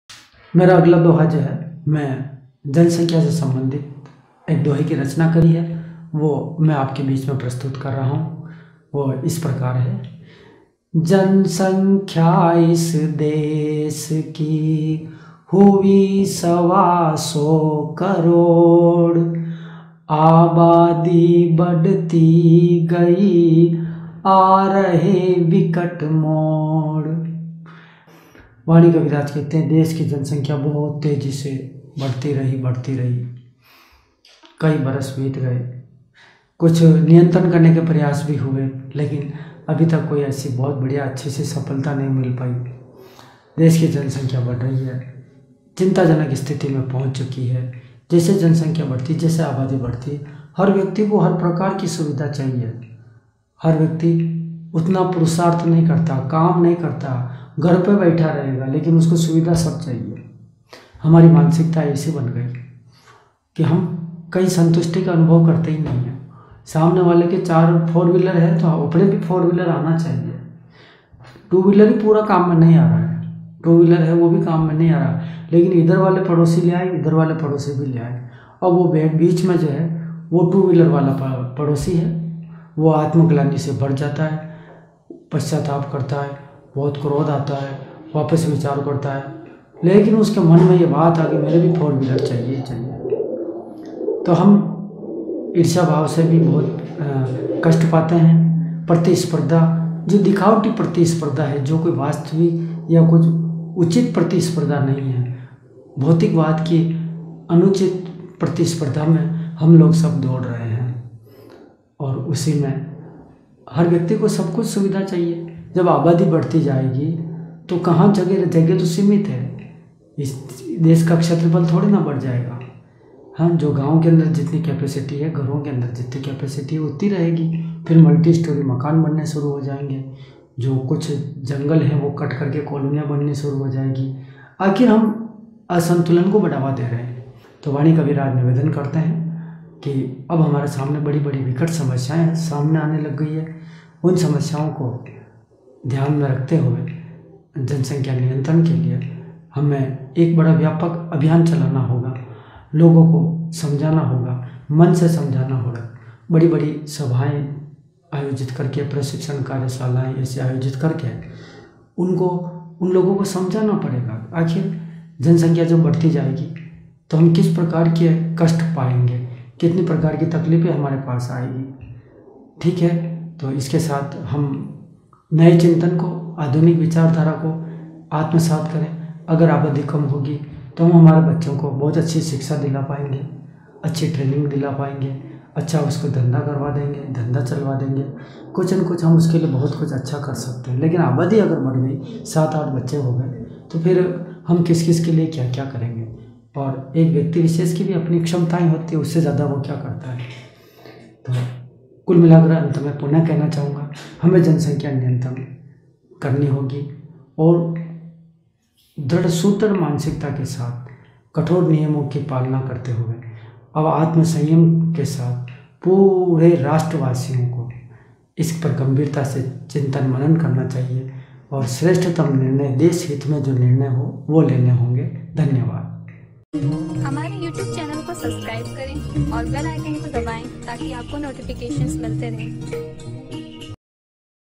Jansankhiya is Desh Ki /जनसंख्या इस देश की - Paryavaran Ke Dohe @ Kavi Amrit 'Wani' (PD85)
Book Name: Paryavaran Par Dohe

जनसंख्या इस देश की, हुई सवा सौ करोड़ ।
आबादी बढ़ती गई, आ रहे विकट मोड़ ।।

© All contents of this Video are protected by copyright By Chetan Prakashan / Chetan Production